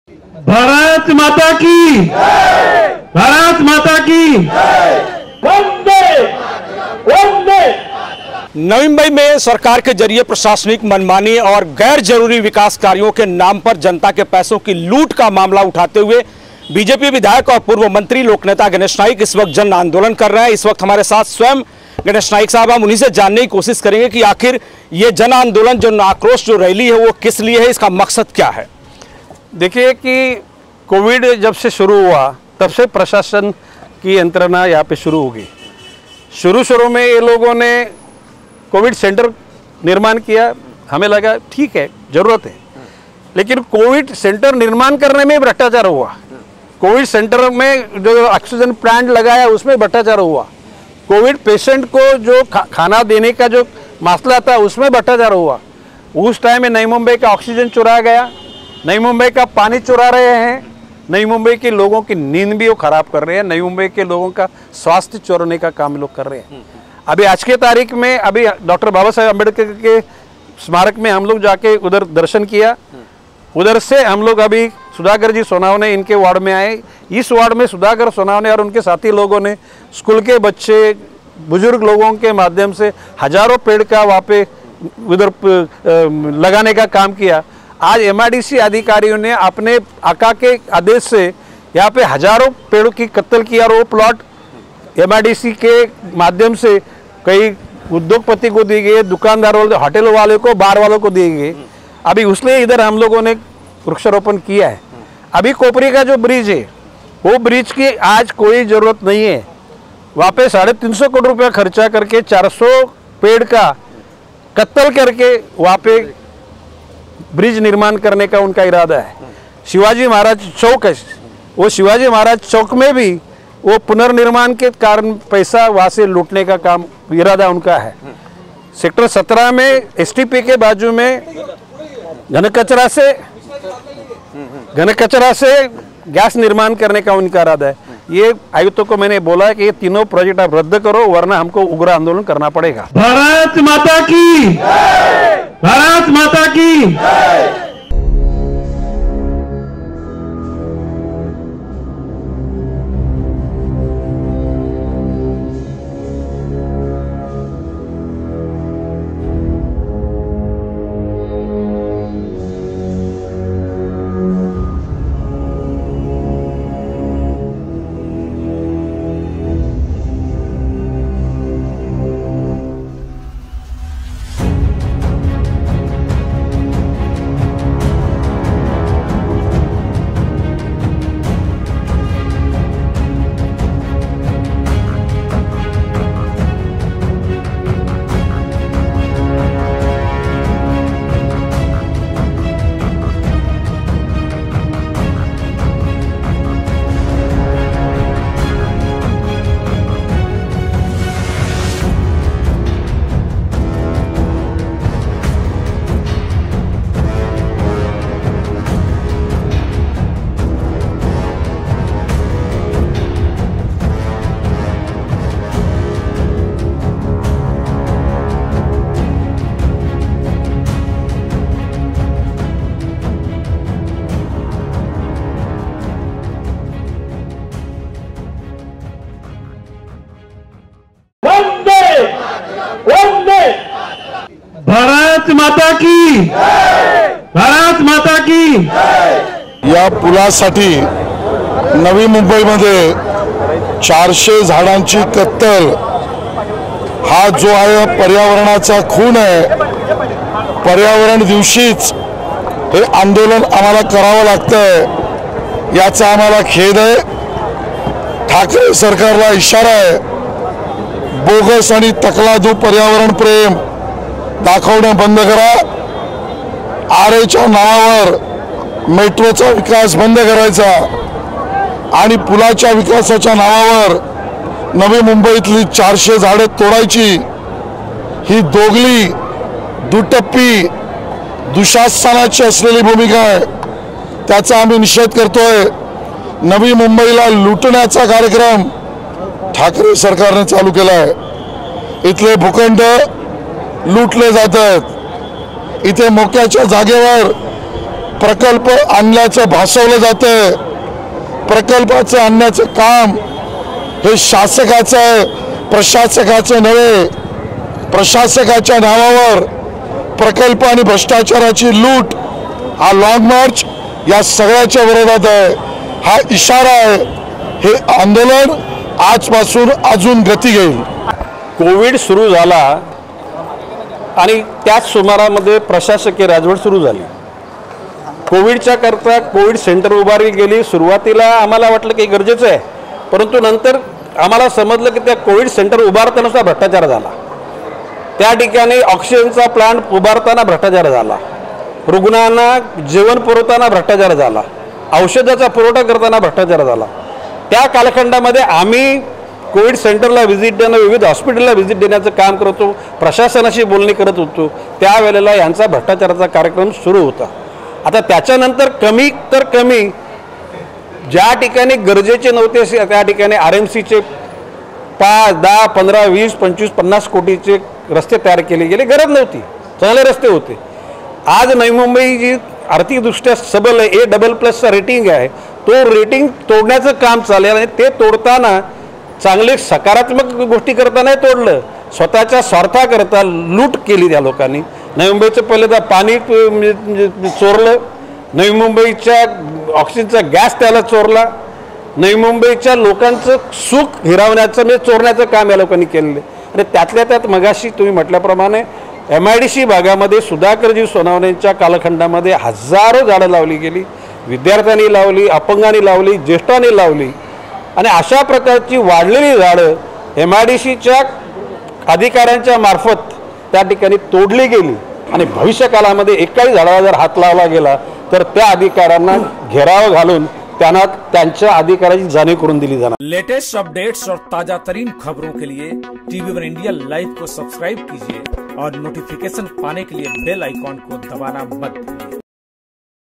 Uddhav सरकार के खिलाफ लांग मार्च | Ganesh Naik का चिपको आंदोलन गरमाया
MLAGaneshNaik का भ्रष्टाचार के खिलाफ लांग मार्च आंदोलन || Tv1India
उद्धव सरकार- मनपा के खिलाफ गणेश नाईक की सिंहगर्जना
भ्रष्टाचार के खिलाफ MLA गणेश नाईक का लांग मार्च आंदोलन
उद्धव सरकार के मंत्री लूट रहे नवी मुंबई-गणेश नाईक
विकास के बहाने नवी मुंबई को नहीं लूटने देंगे-गणेश नाईक